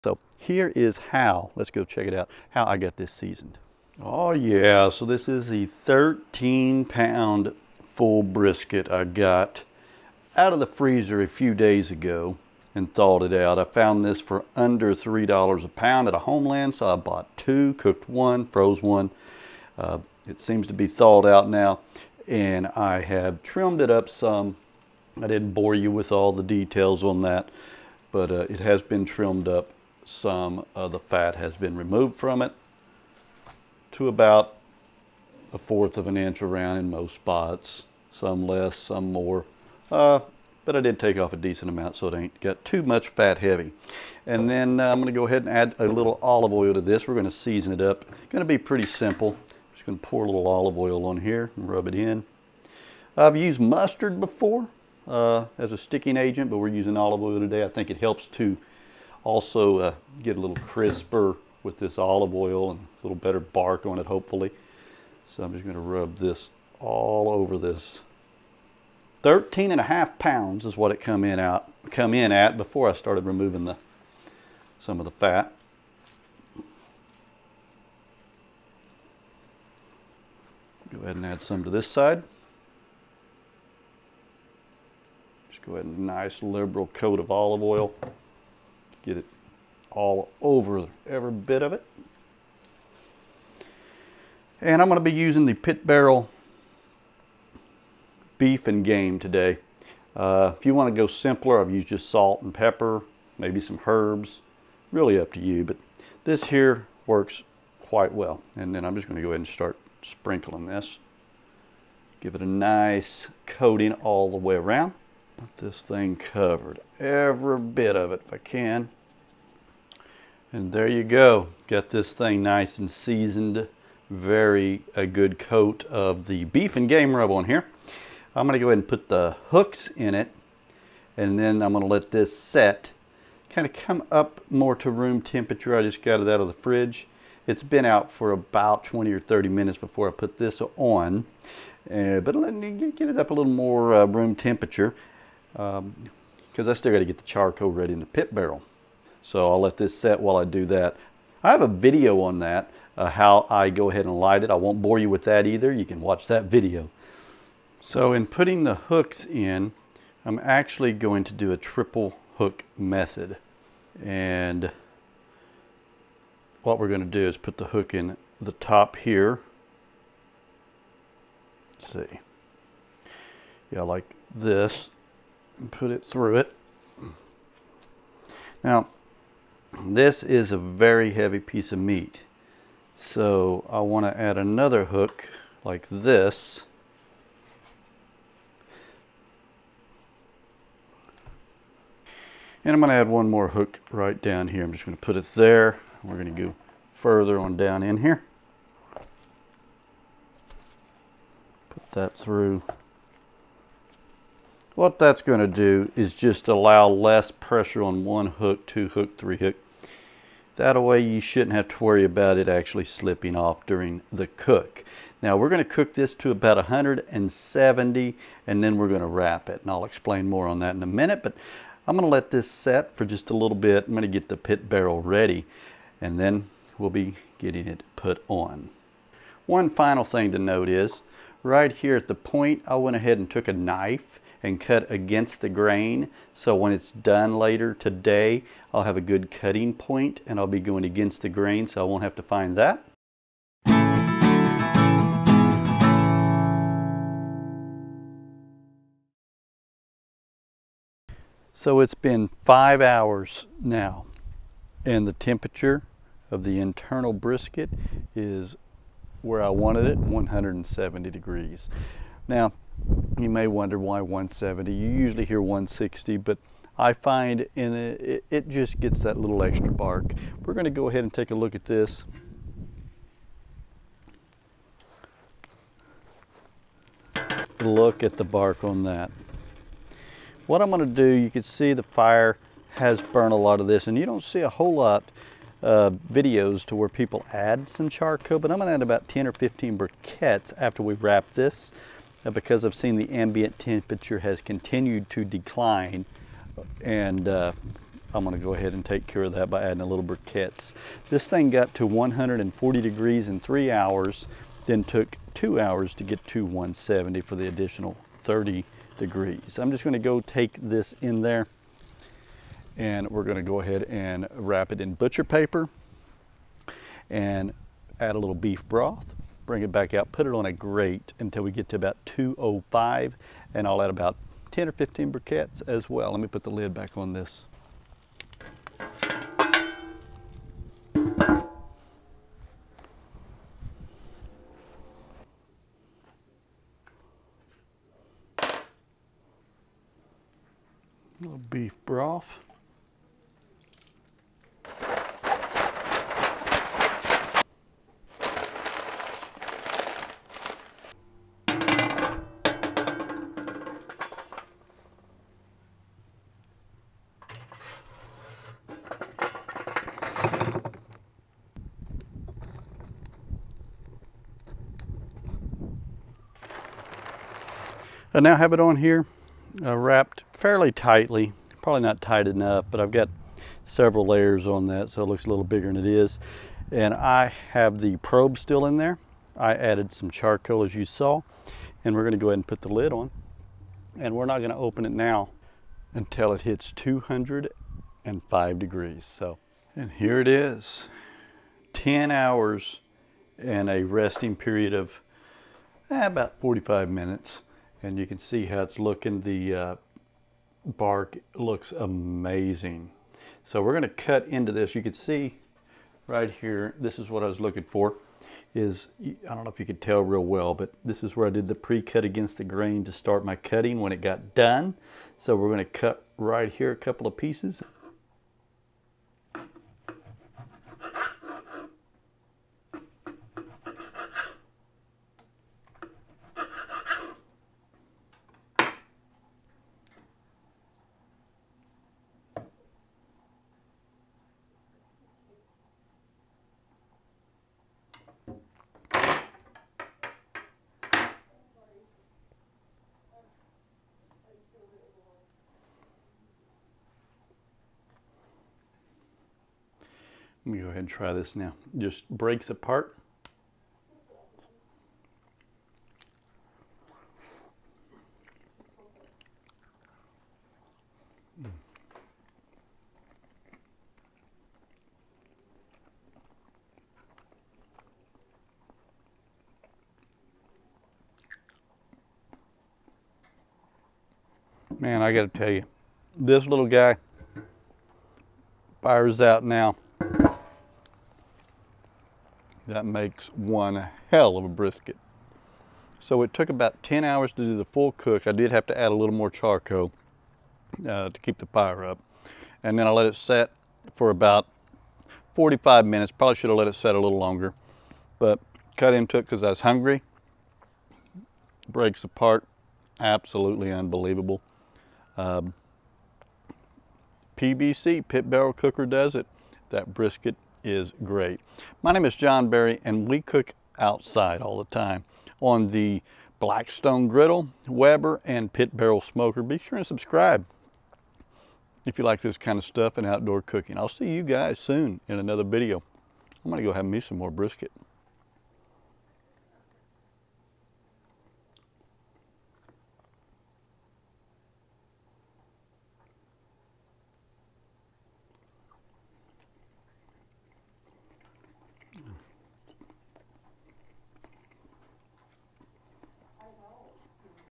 Brisket Smoked In The Pit Barrel Cooker | 13.5 Pounder!
Cooked a beautiful 13.5-pound full-packer brisket on the Pit Barrel Cooker using beef and game rub.  It came out tender and juicy and had an amazing-looking bark.

Ingredients:

• 1 Large Brisket
• Olive Oil
• Seasoning (I used beef and game rub)
• Beef Broth, Beer, or Apple Juice (I used beef broth)

0:00 Intro
0:50 Adding Olive Oil
1:35 Seasoning Brisket
2:37 3 Hook Method
4:03 Cooking The Brisket On The Pit Barrel
5:36 Wrapping The Brisket In Butcher Paper
6:43 Cutting Into The Brisket
7:20 Tasting It